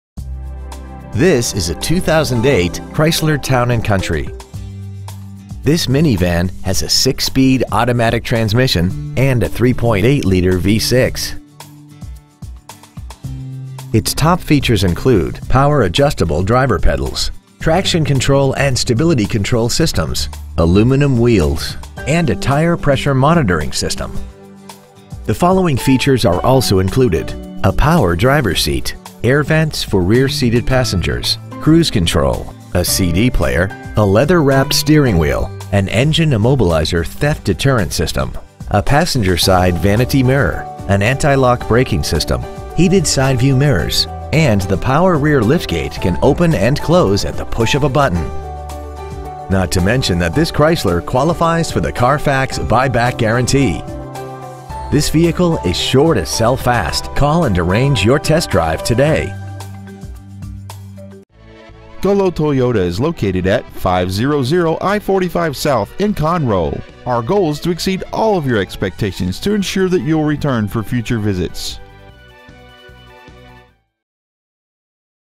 2008 Chrysler Town Country Woodlands TX
Year : 2008
 Make : Chrysler
 Model : Town and Country
 Engine : Gas V6 3.8L/231
 Trans . : Automatic
 Exterior : Modern Blue Pearl
 Miles : 102,227
 Interior : Md Slate Gray/Lt Shale
 Stock : X21182B

 Used 2008 Chrysler Town and Country Conroe Texas